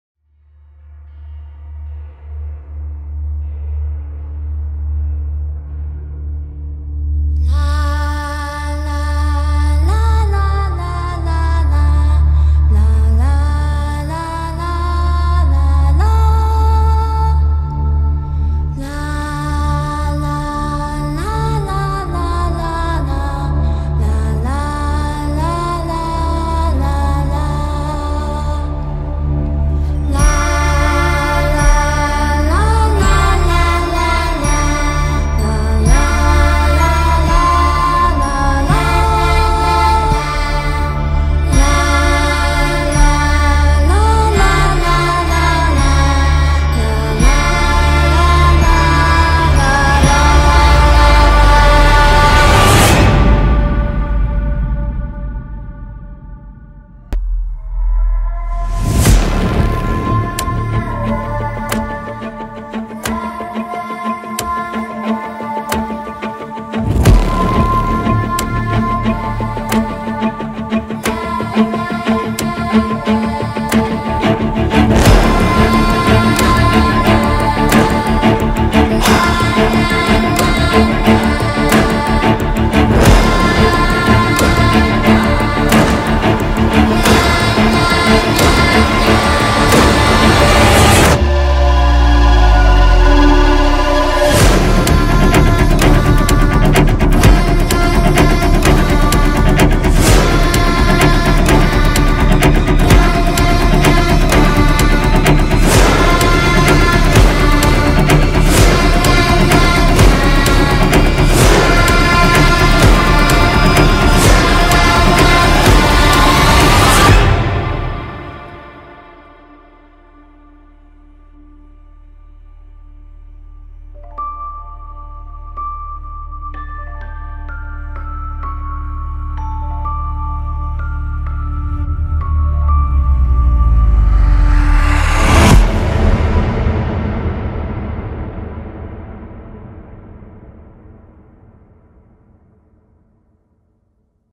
Cinématique au dessus du lac d'Ilay (Jura 2020).
Petite cinématique en drone racer.